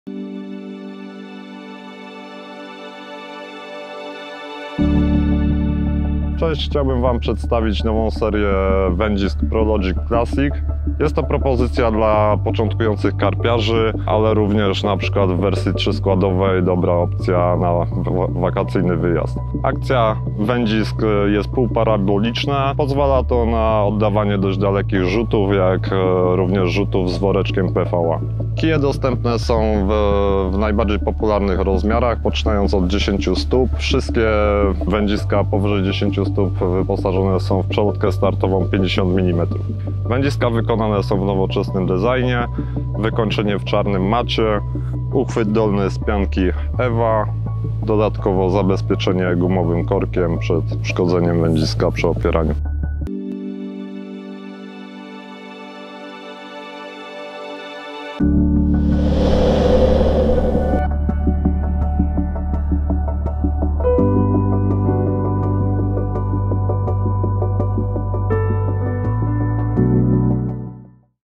Prologic - Wędziska Classic
Sprzęt dla początkującego karpiarza nie powinien on być zbyt drogi, bo na tym pierwszym etapie nie wykorzystamy w pełni jego możliwości, ale nie można poświęcić tu jego jakości i sprawności, bo tani i marny może mieć wpływ na nasze zniechęcenie. Przykładem dobrze wyważonego pod względem stosunku ceny do jakości sprzętu karpiowego dla początkujących są wędziska Prologic Classic. 🎣🎣🎣